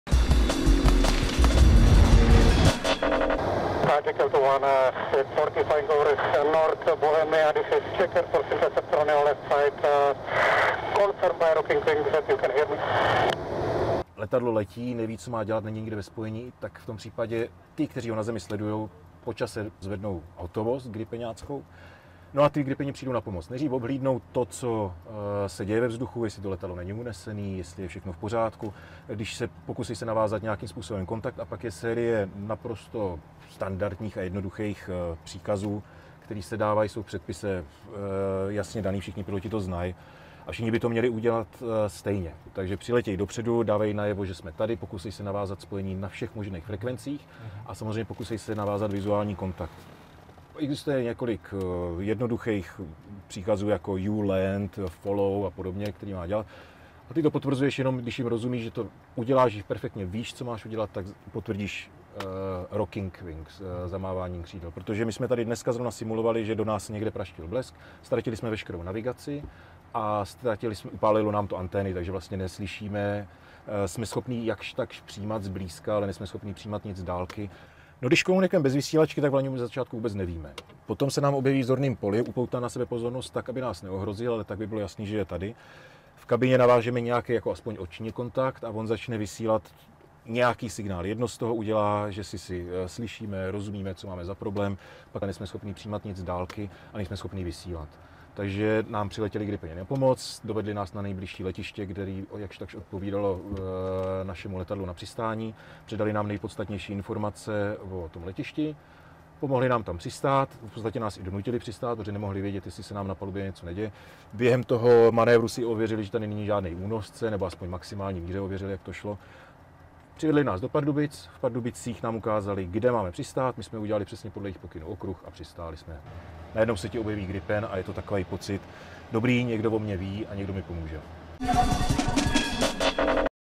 Gripeny vyrazily k mlčícímu JAK 40
Gripeny vyrazily k mlčícímu JAK-40. Cvičný zásah prověřil schopnosti komunikace.
Letouny JAS-39 Gripen hlídají vzdušný prostor České republiky a v případě potřeby startují k nekomunikujícím letounům či letounům v tísni. Právě to byl případ cvičení, při kterém byla prověřena nejen vizuální komunikace, ale i schopnosti rychlé reakce na potenciální hrozbu.

„Dnes se cvičila součinnost s piloty z 21. základny taktického letectva. Prakticky šlo o air policing, pomoc letadlům, které jsou v nouzi či neodpovídají. V našem případě byla simulována závada po zásahu blesku, kdy letoun přišel o navigační a komunikační prostředky. S pomocí dvojice hotovostních gripenů byl letoun doprovozen na přistání na letišti v Pardubicích, tedy nejbližší letiště odpovídající parametry pro přistání letounu JAK-40. Tomu samozřejmě předcházela řada úkonů, využití vizuálních dorozumívacích prostředků, které by měl znát každý pilot. Pro jakéhokoliv pilota v nouzi znamená přiblížení gripenů znamení, že někdo o nás ví, je to dobré, někdo mi pomůže," uvedl major Ondřej Kudělka, pilot letounu JAK-40 z 24. základny dopravního letectva v pražských Kbelích.

A jak takový ostrý zásah vypadá a kdo k němu vydává rozkaz? Čáslavská základna vyčleňuje do nepřetržité hotovosti v rámci NATINAMDS (Integrovaný systém protivzdušné a protiraketové obrany států Severoatlantické aliance) dva víceúčelové nadzvukové letouny JAS-39 Gripen a jeden letoun jako zálohu. Stroje disponují odpovídajícím zbraňovým vybavením. V případě, že dojde k narušení vzdušného prostoru, případně pokud letoun neodpovídá, či vykazuje další, zvláštní znaky, jako je kupříkladu změna kurzu atp., v ten okamžik dostávají hotovostní piloti jasný povel k okamžitému vzletu Alfa Scramble.

Signál alfa představuje vzlet do patnácti minut. Čáslavští stíhači to obvykle zvládnou ještě před limitem. Cvičný vzlet se nazývá Tango Scramble a piloti při něm musí prokázat svoji profesní připravenost. V tomto případě povel k okamžitému vzletu byl doprovázen informací o narušiteli. Tím se stal zmíněný letoun JAK-40, se kterým se nepodařilo navázat spojení a který se od příhraničí pohyboval dále do vnitrozemí. Po vzletu bylo s letounem navázáno během několika minut vizuální spojení, při kterém dvojice letounů zevrubně kontroluje nejen stav letounu, tedy potenciální hrozby, ale také se snaží monitorovat situaci na palubě, pokud by šlo například o únos. Během roku se odehraje celá řada podobných vzletů, zatím vždy kvůli závadám či pochybení pilota.